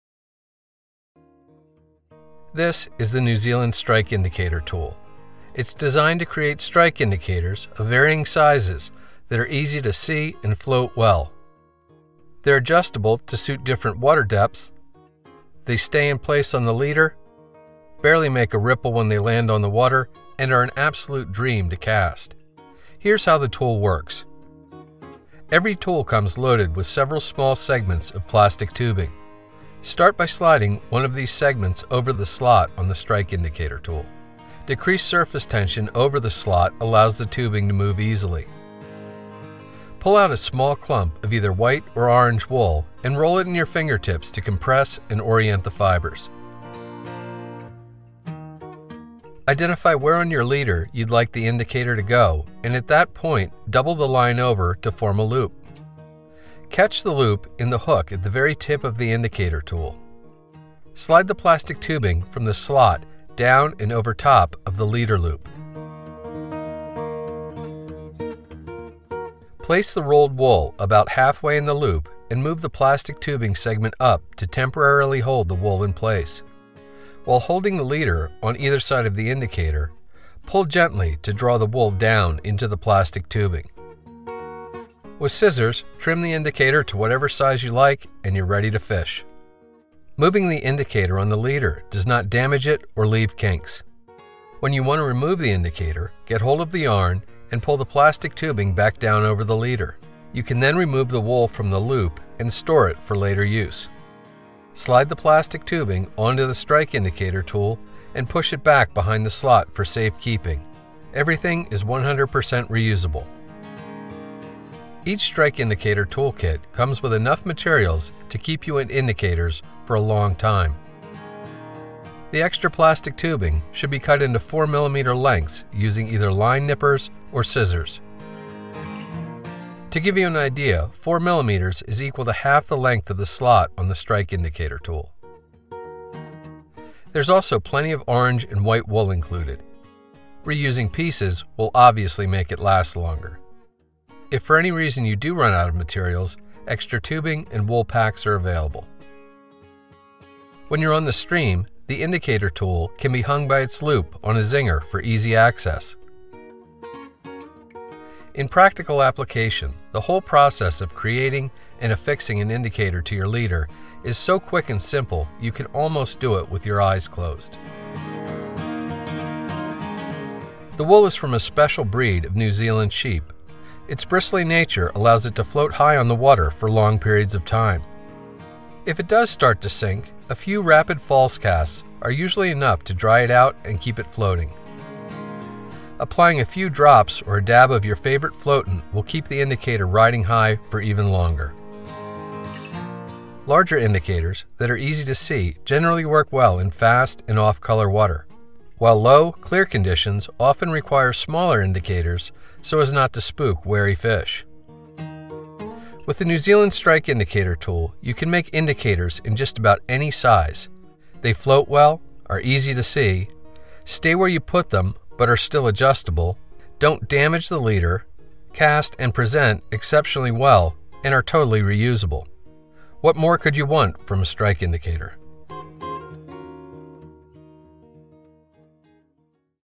New Zealand Strike Indicator Tool
New Zealand anglers fish in some of the most difficult conditions on the planet. What better place for design and testing a new strike indicator system that gives you more than one chance for a delicate presentation at the proper depth to hook a trophy trout. From tiny mountain stream to big rivers and lakes, more than 20 New Zealand professional guides tested this system with their customers in all types of weather and water and found it to be a winner. You will, too!